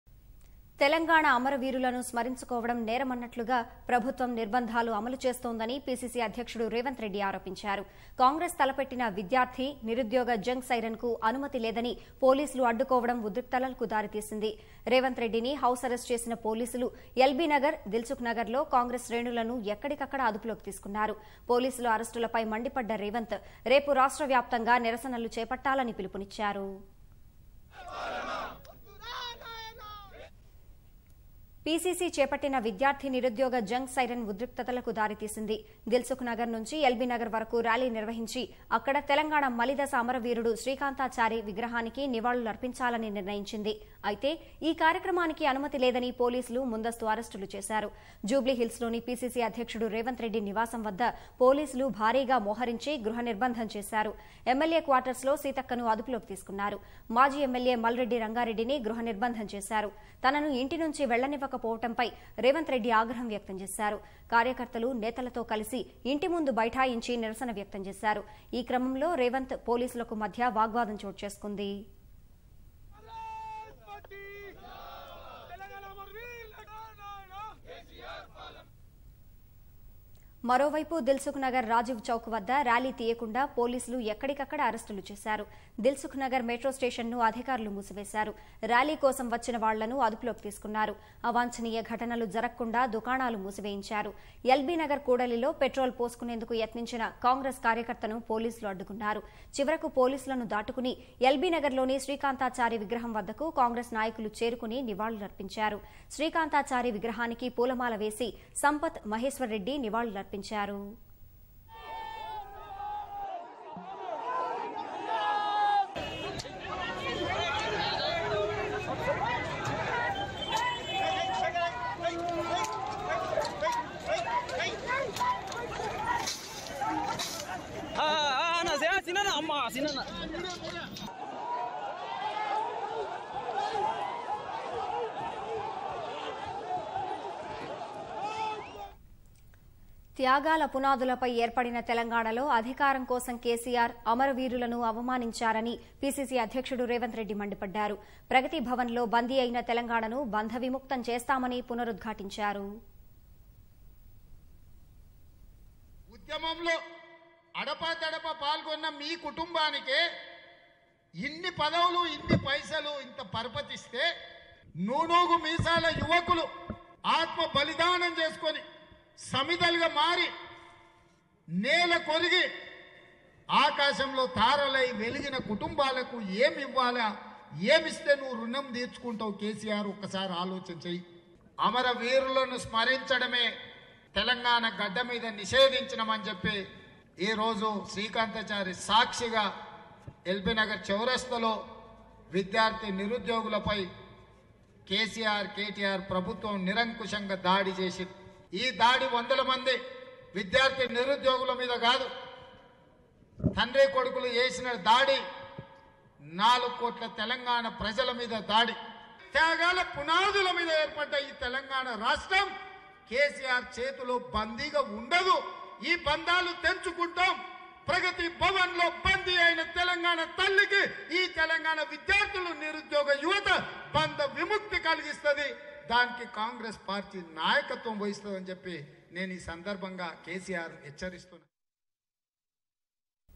ర్యాలీ అడ్డుకోవడాన్ని తప్పు పట్టిన రేవంత్‌ రెడ్డి | Revanth Reddy On Police Obstructing the Rally
తెలంగాణ అమరవీరులను స్మరించుకోవడం నేరమన్నట్లుగా ప్రభుత్వం నిర్బంధాలు అమలుచేస్తోందని PCC అధ్యక్షుడు రేవంత్ రెడ్డి ఆరోపించారు. కాంగ్రెస్  తలపెట్టిన విద్యార్థి, నిరుద్యోగ జంగ్  సైరన్ కు అనుమతి లేదని పోలీసులు అడ్డుకోవడం ఉద్రిక్తతలకు  దారితీసింది. రేవంత్ రెడ్డిని హౌస్  అరెస్టు చేసిన పోలీసులు.. ఎల్బీనగర్ , దిల్ సుఖ్ నగర్ లో కాంగ్రెస్  శ్రేణులను ఎక్కడికక్కడ అదుపులోకి తీసుకున్నారు. పోలీసుల అరెస్టులపై మండిపడ్డ రేవంత్ ... రేపు రాష్ట్రవ్యాప్తంగా నిరసనలు చేపట్టాలని పిలుపునిచ్చారు.